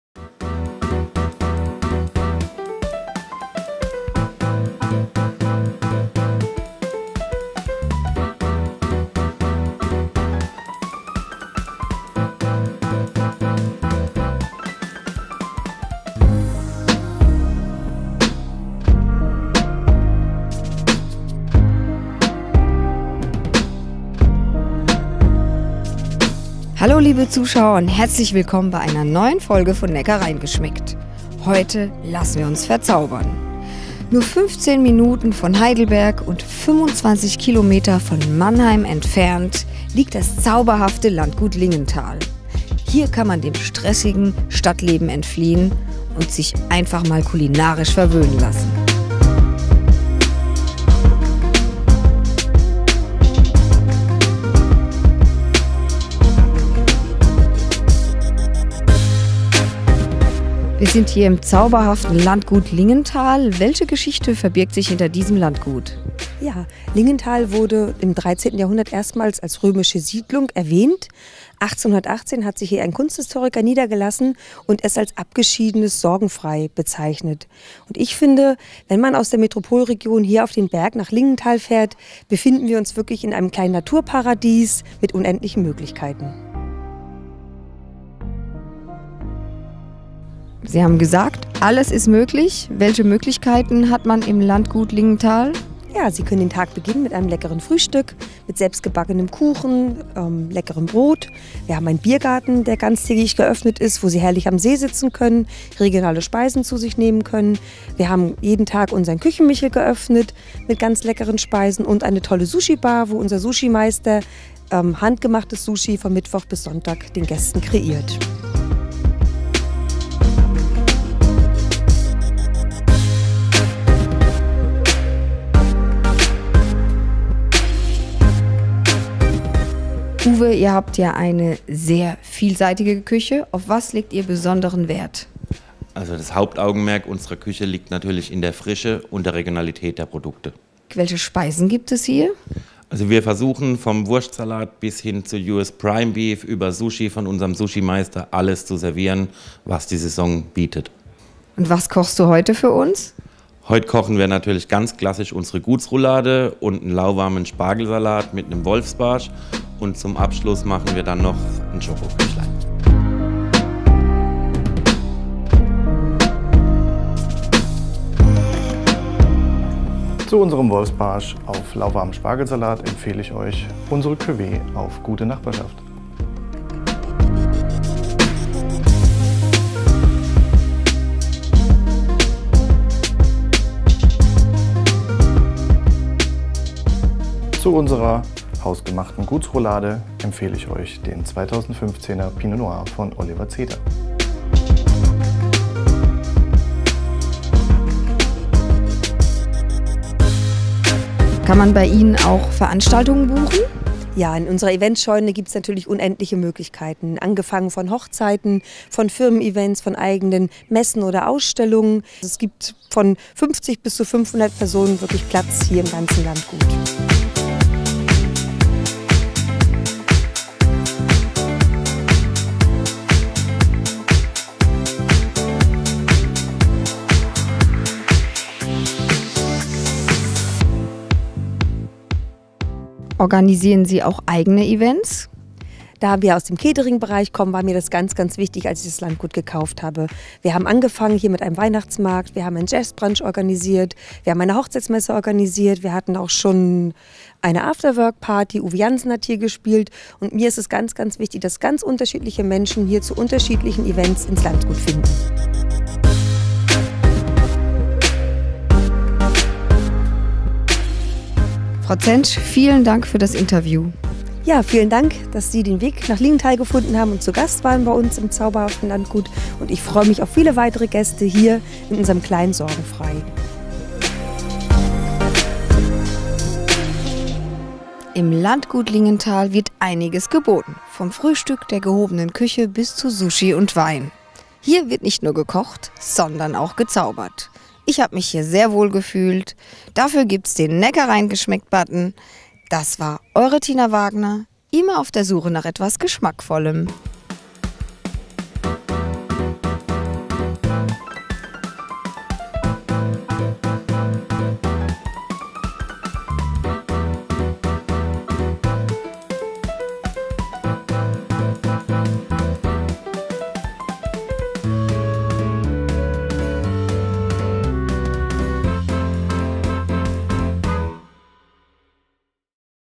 Zauberhaftes Landgut Lingental
Liebe Feinschmecker, heute entführen wir Euch in das Zauberhafte Landgut Lingental. 25 Kilometer von Mannheim und nur 15 Autominuten von der Heidelberger Altstadt entfernt liegt das Landgut Lingental. Der 1,3 Hektar Grundfläche umfassende Hof ist das historische Kerngut
des Ortes, der erstmals 1312 als „Langental“ Erwähnung fand. Ob Frühstück mit leckerem Kuchen im "Landcafe" oder Mittagessen im "Küchenmichel" bis hin zum Abendessen im historischen Dachgewölbe mit "Sushi &Wein" das Landgut Lingental hat alles was das kulinarische Herz begehrt.